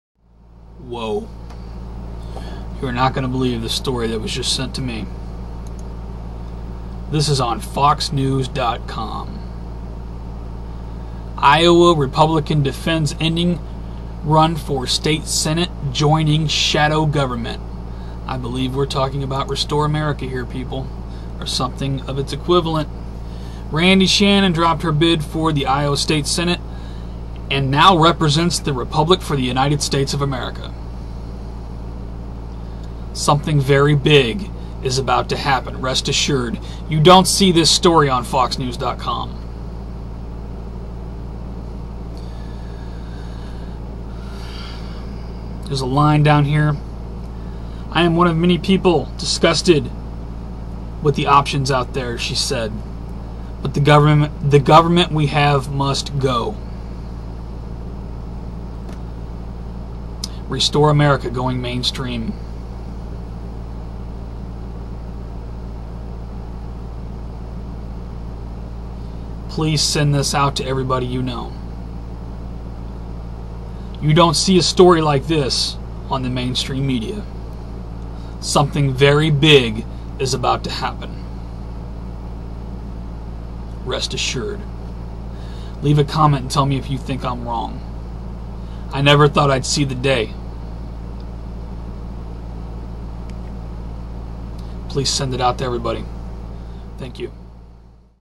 BREAKING - Iowa Republican Joins Shadow Government
PLEASE MAKE VIRAL!

Iowa Republican Defends Ending Run For State Senate Joining Shadow Government

An Iowa Republican has caused a stir after ending her run for state legislature, and then becoming a "senator" for a de facto shadow government known as the Republic for the United States of America.

The former candidate, Randi Shannon, defended the bizarre decision in an interview with FoxNews.com on Monday, and did her best to explain her reasons for doing so.

"I am one of many people so disgusted with the options out there," she said. "But the government we have has to go."

Organizers say the Republic for the United States of America was the original government in this country, before it was replaced 225 years ago by the "de facto corporation known as the United States."

My Links

--- U.S. Scancast Website ---

U.S. Scancast Nationwide Scanner Network
Live Nationwide Scanner Audio And Chat 24/7